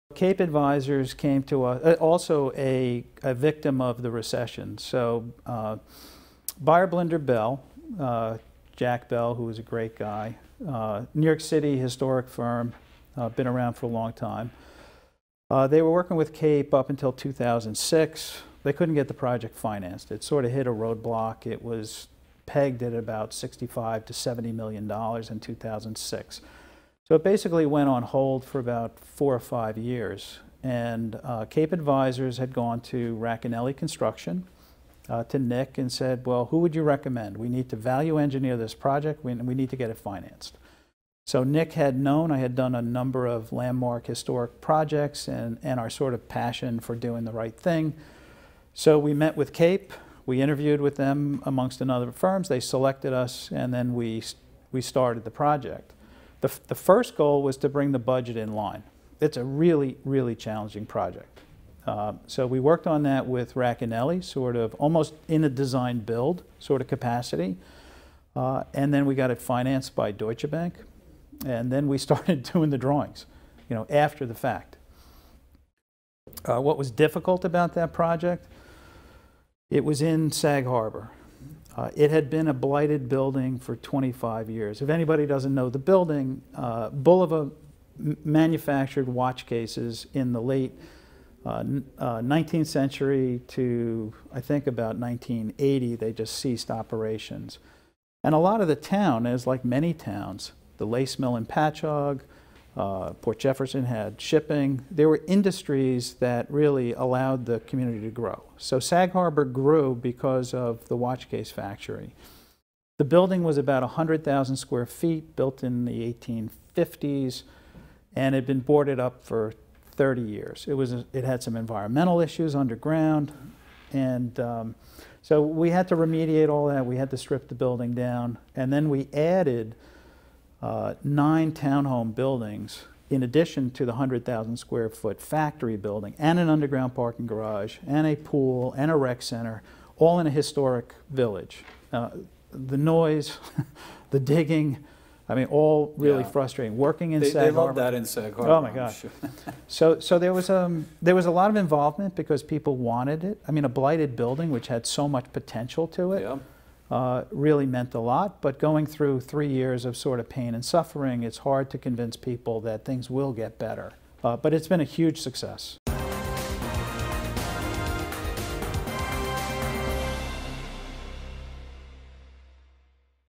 Alex Badalamenti on Restoring Bulova Watchcase Factory
CMM Managing Partner Joe Campolo and bld architecture President/CEO Alexander Badalamenti, AIA, discuss bld architecture’s process as they worked to restore the historic Bulova Watchcase Factory in Sag Harbor.